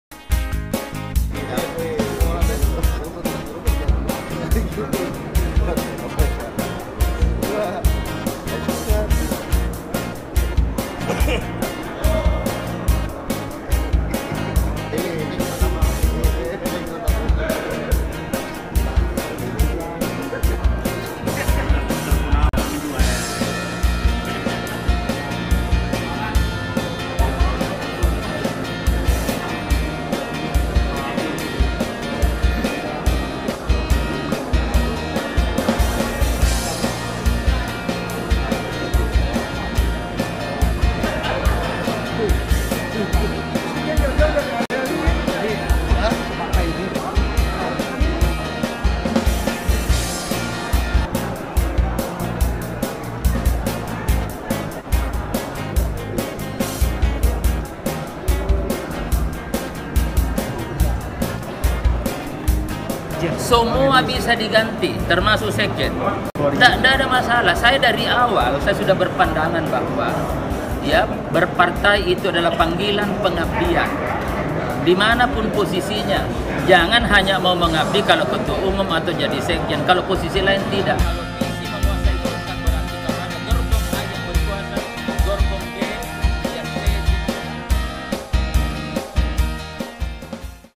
Idrus Siap Jika Dicopot dari Jabatan Sekjen Partai Golkar
Idrus Marham selaku Sekjen Partai Golkar mengaku siap jika harus dicopot dari jabatannya. Hal ini berkaitan dengan wacana Airlangga Hartarto melakukan revitalisasi pengurus partai.